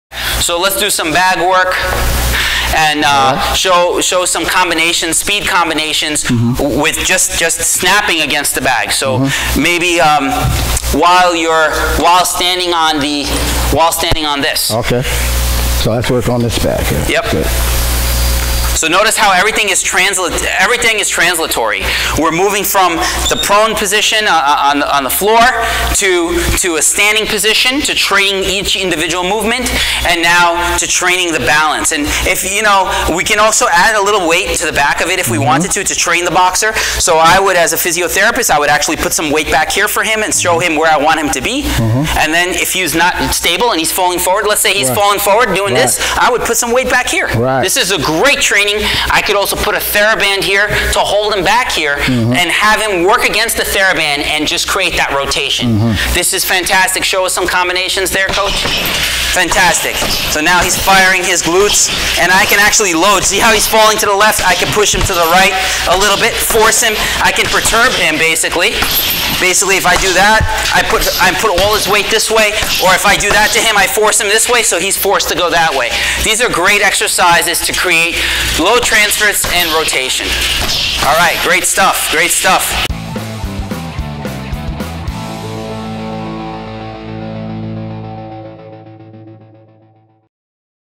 HOW TO NOT BOX IN PAIN - Top Boxing drills Bag Training on a Bosu Ball
PainFree Maverick - Boxing Series
Make sure not to miss a single video from the PainFree Maverick!